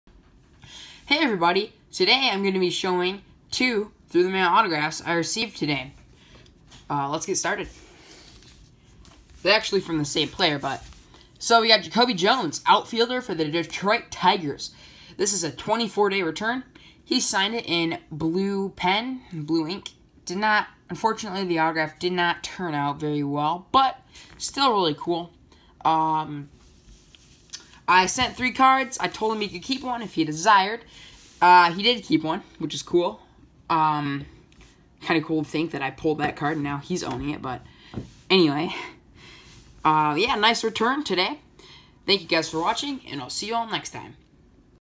Jacoby Jones Autograph TTM!
Hi! Today I will be showing two through the mail autographs I received today in the mail. Thanks!